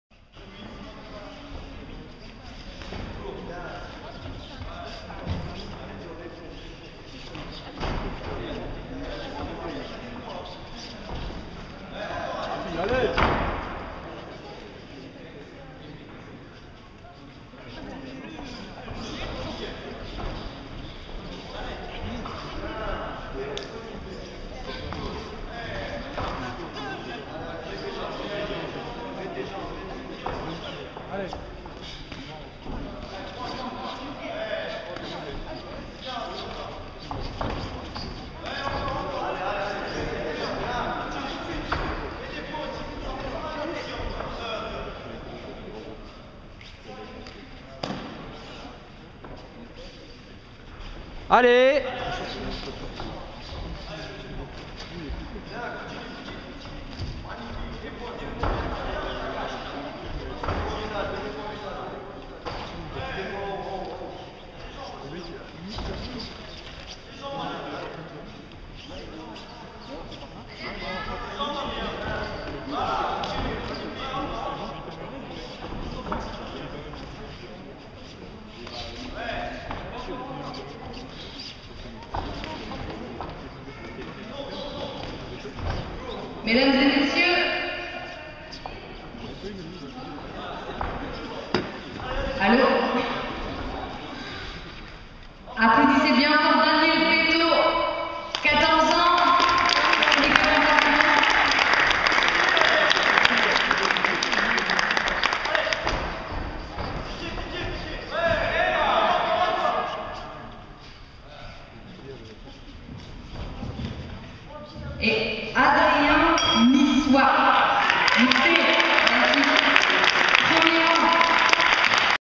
FCS V - Adrien vs Daniel - round 1
1er round du combat d'Adrien (20 ans / pantalon noir) et Daniel (14 ans / pantalon blanc) lors de la Full Contact Session V, le 28 mai 2011 à Guyancourt

Boxe américaine - light contact - moins de 71 kg - 3 rounds de 2 minutes

Victoire d'Adrien